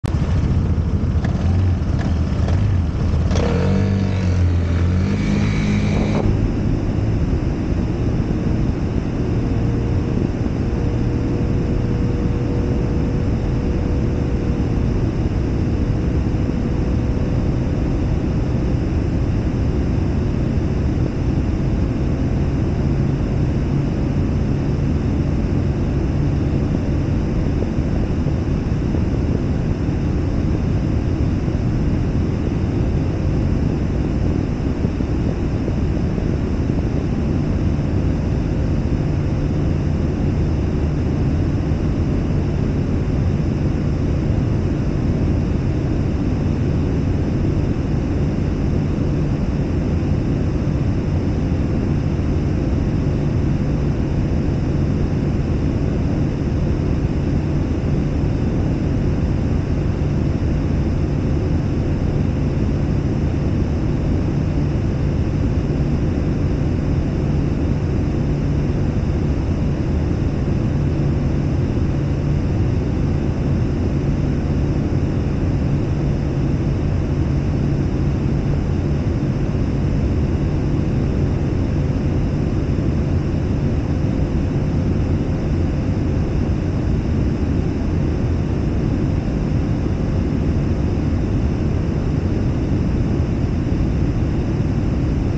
[1K] Vespa GTS300 sport MY2015 with full throttling
hey, I bought new Insta360 OneX2.
It's super easy to manipulate as well as straightforward to post-processing.
It's near fool-proof I guess.

It is pilot video, and I will upload video taken where I live.
If you like this video, please give me a generous thumb up!